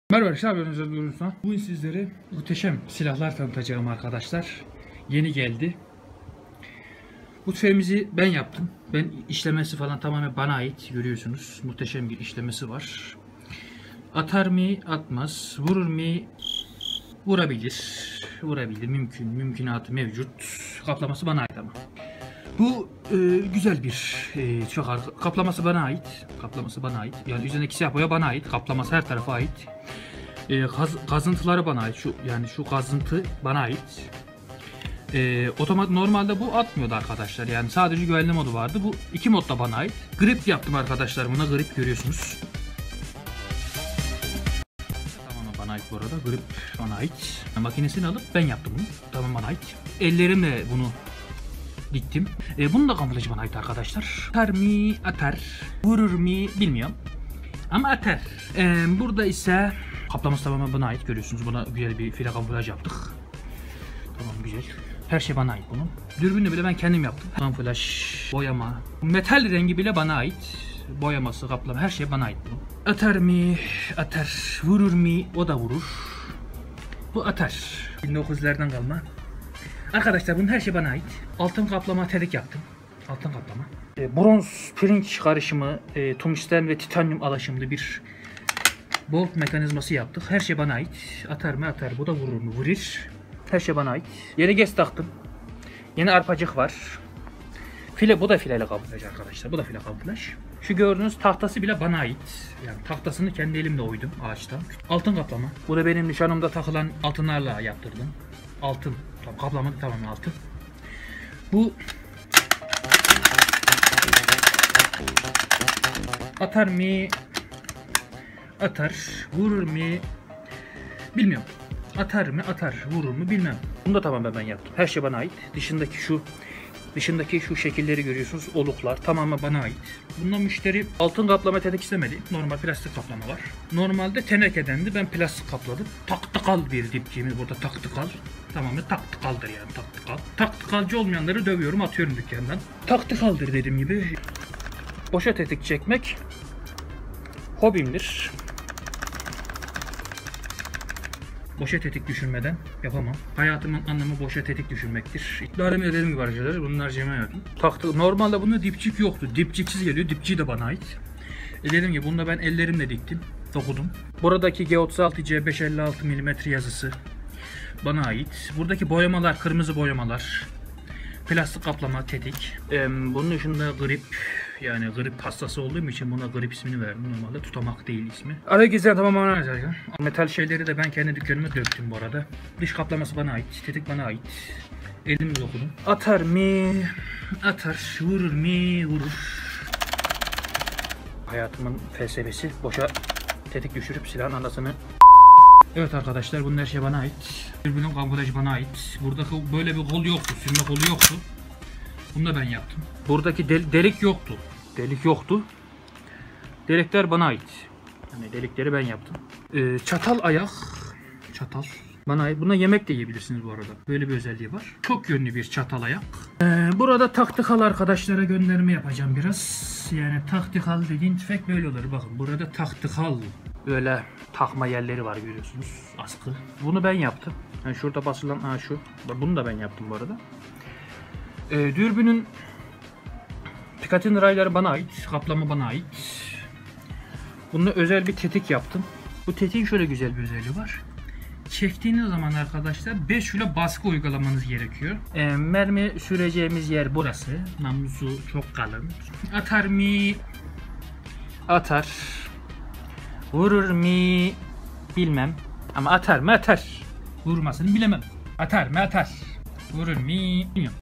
Silahçı Dursun Usta Taklidi (Atar Mi Atar)
Sevdiğim youtube kanalı olan "SİLAHÇI DURSUN USTA'NIN" Kendi eklentilerim ile birlikte ironi amaçlı taklidi.